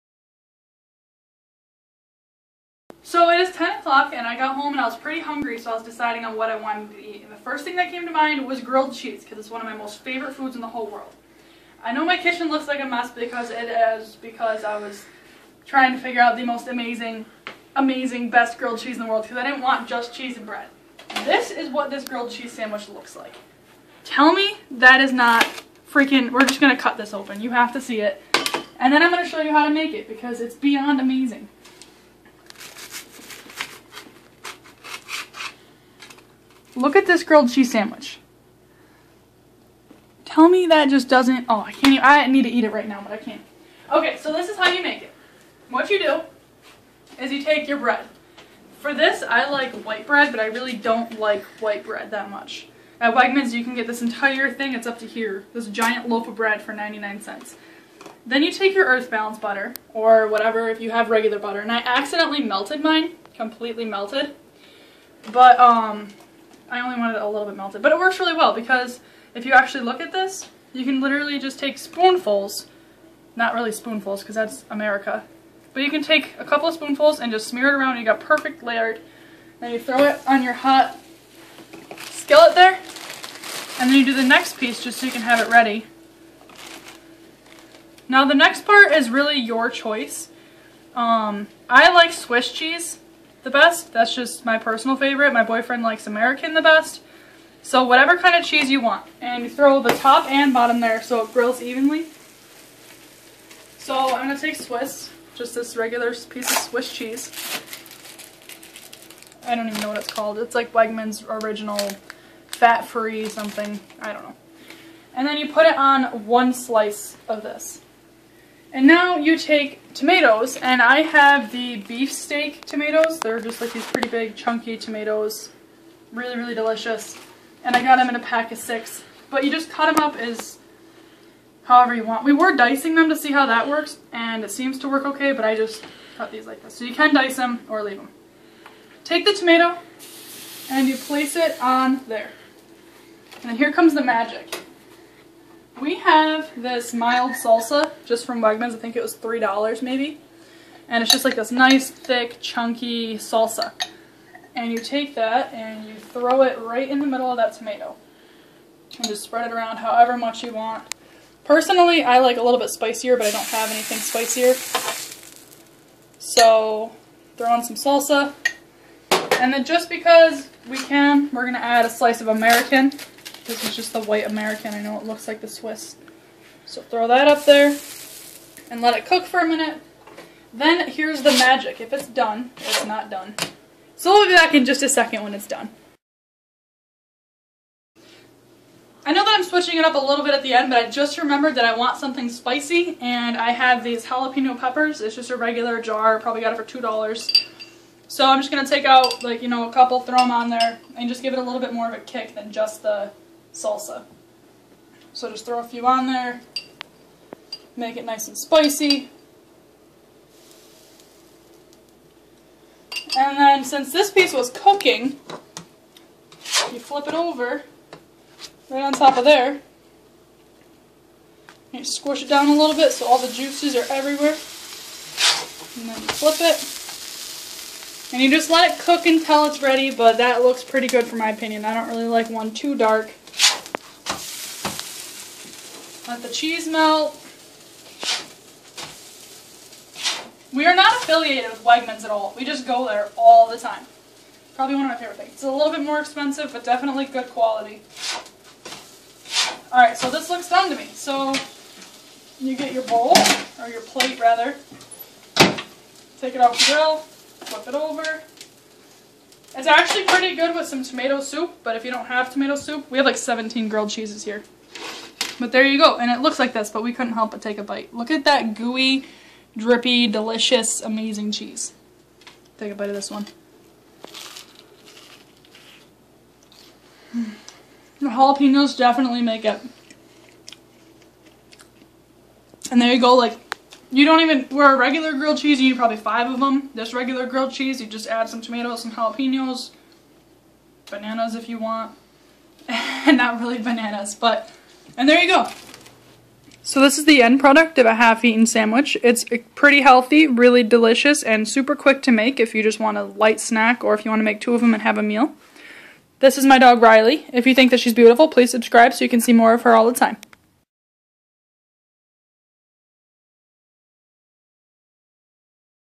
The most amazing grilled Cheese
I got home late from work, I was in school all day- I was just hungry, I threw this together and loved it! So I had to share it with you!